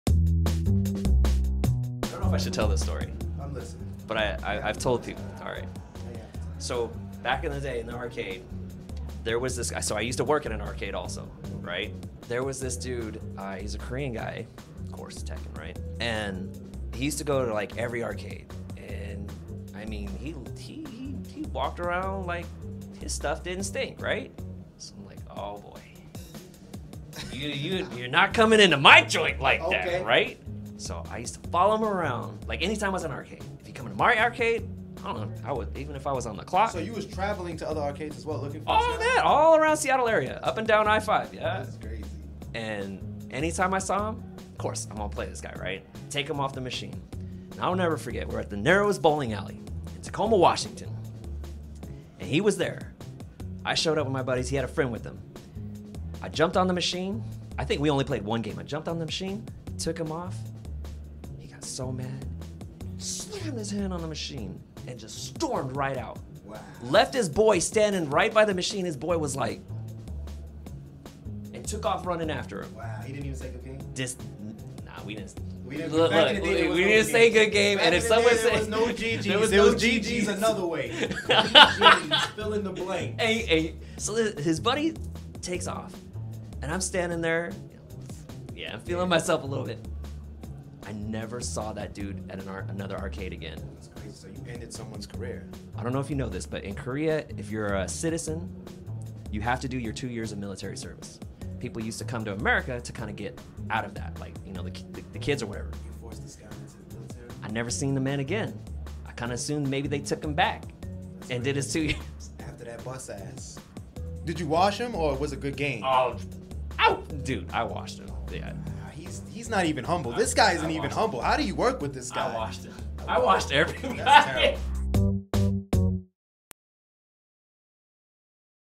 FGC Stories - How Michael Martin might've sent a man to the military because of Tekken
Michael Martin recalls the legendary story of being such a terror in the Seattle Tekken scene, that he might have sent a man to the military.